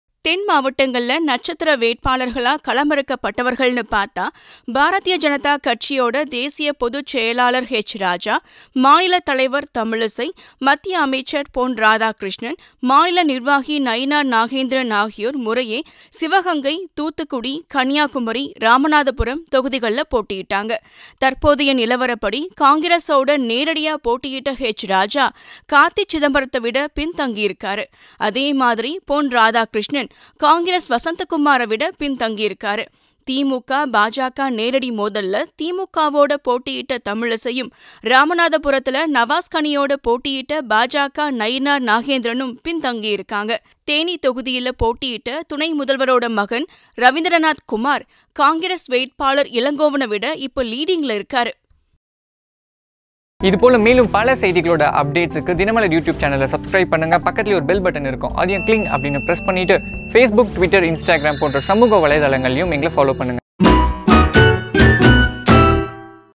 தென்மாவட்டங்களில் பின்தங்கிய பா.ஜ தலைவர்கள்
தென்மாவட்டங்களில் நட்சத்திர வேட்பாளர்களாக களமிறக்கப்பட்டவர்கள்னு பார்த்தால், பாரதிய ஜனதா கட்சியின் தேசிய பொதுச் செயலாளர் எச். ராஜா, மாநில தலைவர் தமிழிசை, மத்திய அமைச்சர் பொன்ராதாகிருஷ்ணன், மாநில நிர்வாகி நயினார் நாகேந்திரன் ஆகியோர் முறையே சிவகங்கை, தூத்துக்குடி, கன்னியாகுமரி, ராமநாதபுரம் தொகுதிகளில் போட்டியிட்டனர்.
தற்போதைய நிலவரப்படி காங்கிரசோட நேரடியா போட்டியிட்ட எச். ராஜா, கார்த்தி சிதம்பரத்தை விட பின்தங்கியிருக்கார். அதேபோல பொன்ராதாகிருஷ்ணன், காங்கிரஸ் வசந்தகுமாரை விட, பின்தங்கியிருக்கார்.
திமுக, பாஜக நேரடி மோதல்ல திமுகவோட போட்டியிட்ட தமிழிசையும், ராமநாதபுரத்துல நவாஸ்கனியோட போட்டியிட்ட பாஜக நயினார் நாகேந்திரனும் பின்தங்கியிருக்காங்க. தேனி தொகுதியில் போட்டியிட்ட துணைமுதல்வரோட மகன் ரவீந்திரநாத் தாகூர், காங்கிரஸ் வேட்பாளர் இளங்கோவனை விட இப்ப லீடிங்க்ல இருக்கார்.